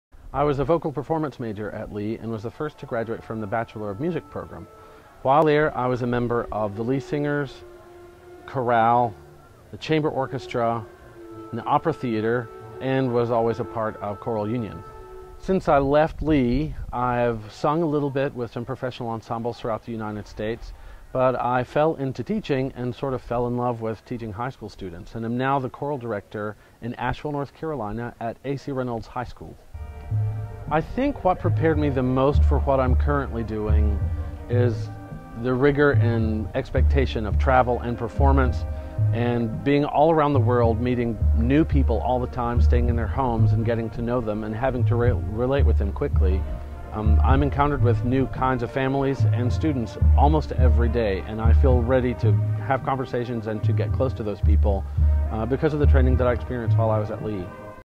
Phillip Haynie
Hear from Lee University School of Music alum, Phillip Haynie, about his time at Lee.